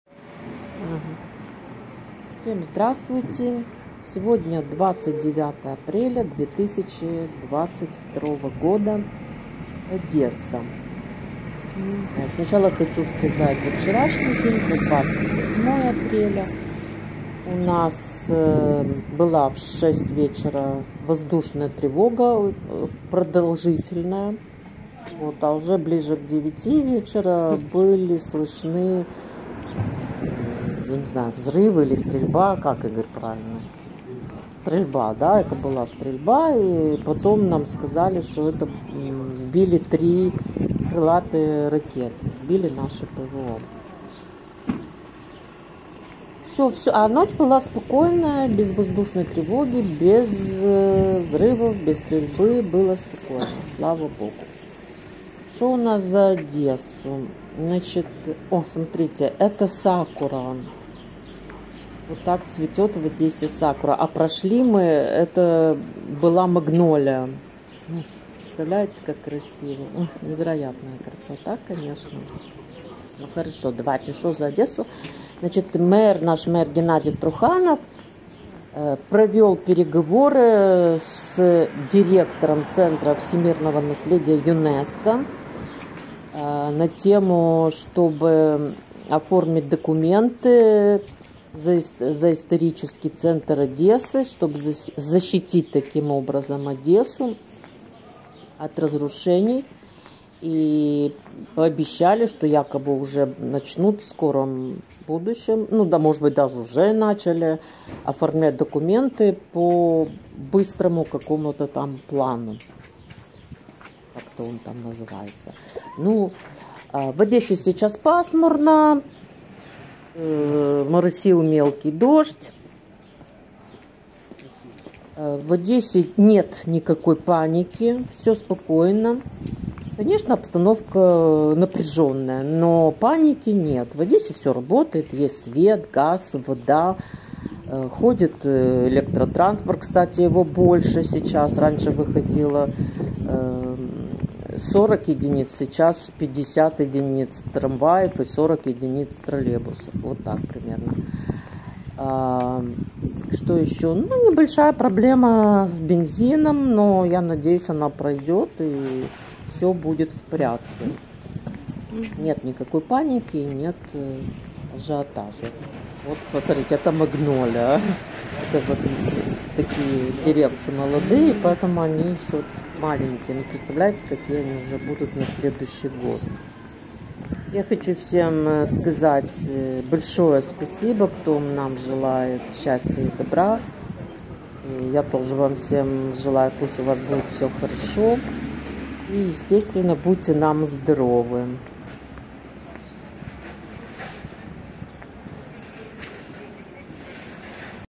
Одесса. 29 апреля 2022.
Сегодня пасмурно, пошел дождь. На улице +12.

Видео Ирина Синюкова.